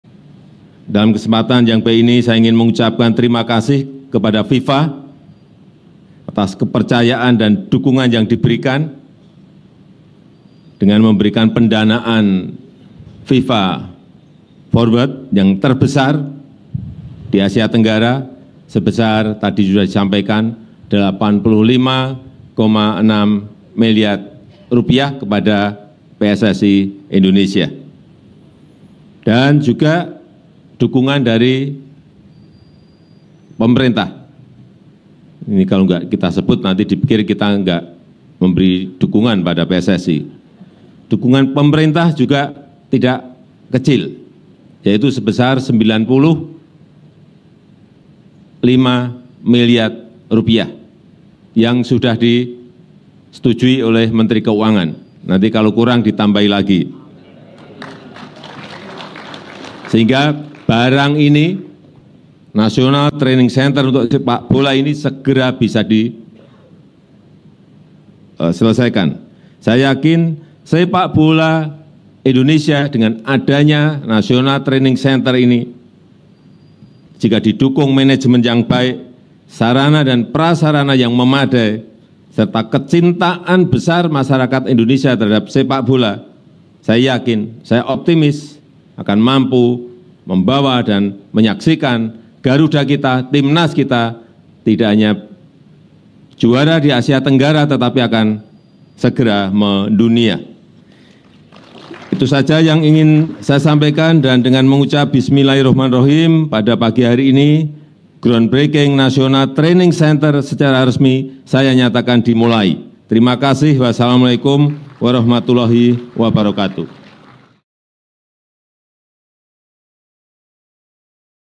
Jokowi Optimis Timnas Indonesia Bakal Mendunia | Liputan6
Liputan6 - Jokowi Optimis Timnas Indonesia Bakal Mendunia | Liputan6

Presiden Joko Widodo melakukan groundbreaking atau peletakan batu pertama sebagai tanda dimulainya pembangunan National Training Center di Ibu Kota Nusantara (IKN), Kalimantan Timur. Pembangunan National Training Center ini diharapkan dapat menunjang proses latihan dan mengembangkan kemampuan Tim Nasional (Timnas) sepak bola Indonesia, Jokowi optimis hal ini dapat membawa Timnas segera mendunia.